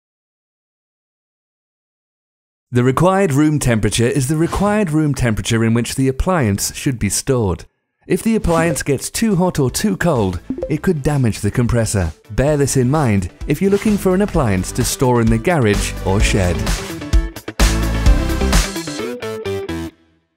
Required Room Temperature for Appliances
The Required Room Temperature is the required room temperature in which the appliance should be stored. If the appliance gets too hot or too cold it could damage the compressor, so bear this in mind if you are looking to store the appliance in the garage or shed.